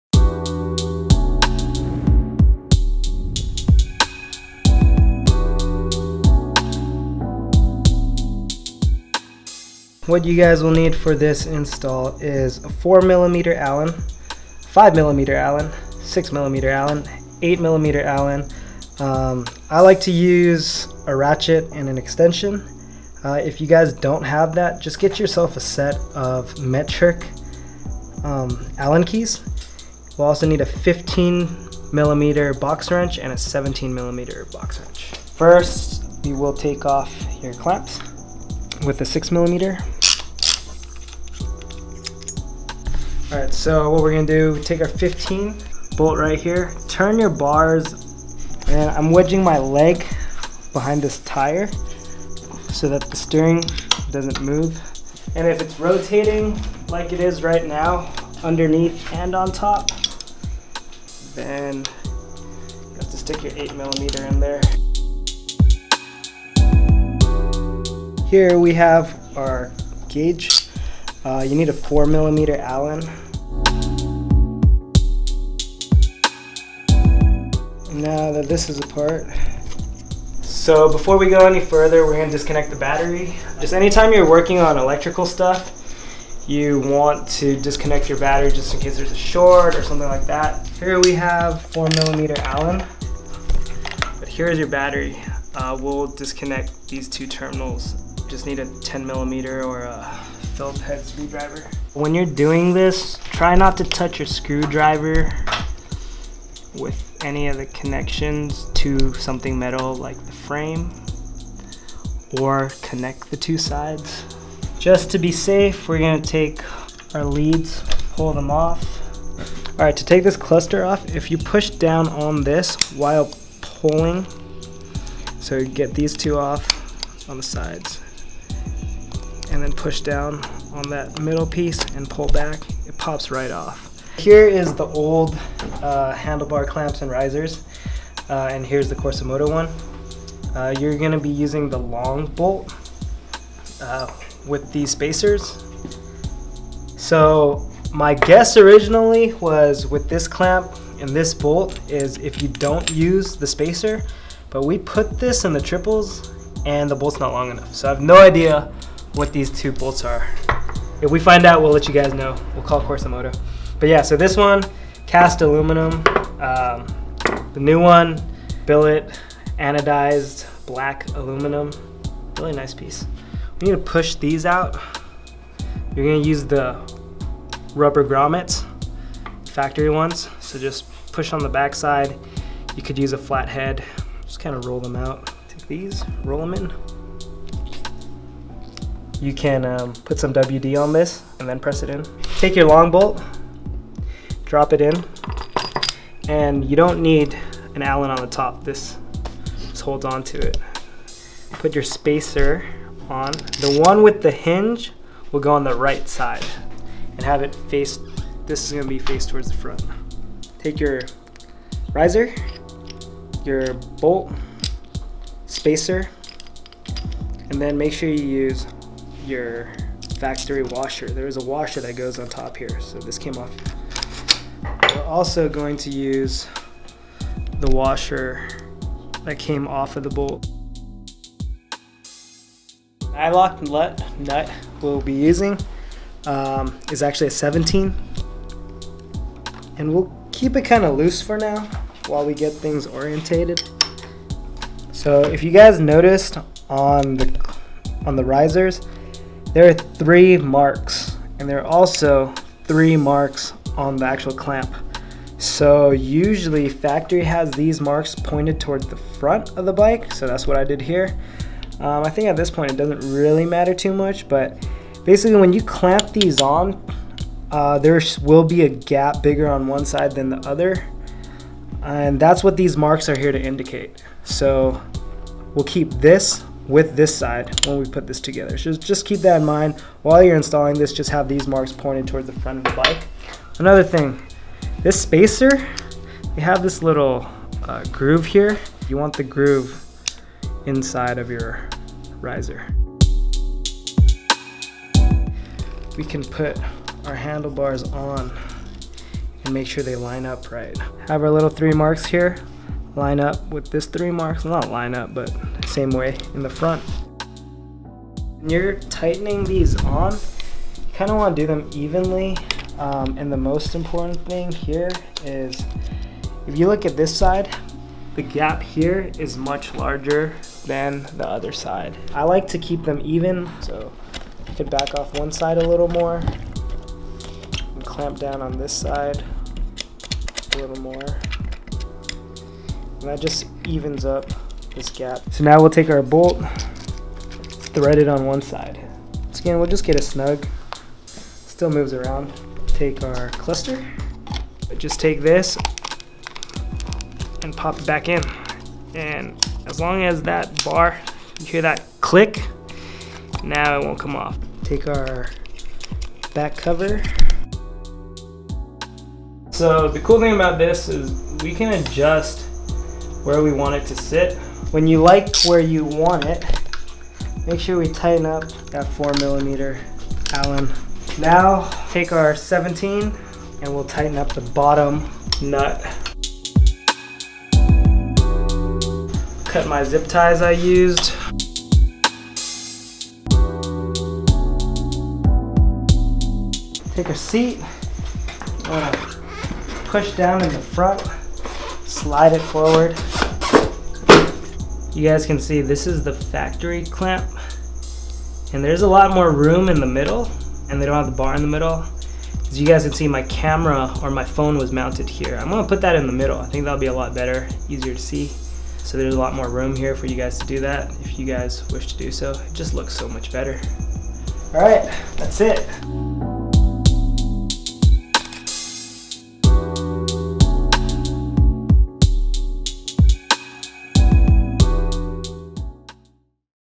Corsa Moto Riser and Gauge Kit Install on Ducati Scrambler
From cheap cast OEM riser, to hard anodized billet aluminum riser that adds more real estate for a variety of mounts! Can change from 7/8" to 1 and 1/8" tapered bars for vice versa. Hope this helps your Install!

Check here to check out more parts for you Scrambler for Corsa Moto -

Shot and edited by @johnmwu follow him on IG!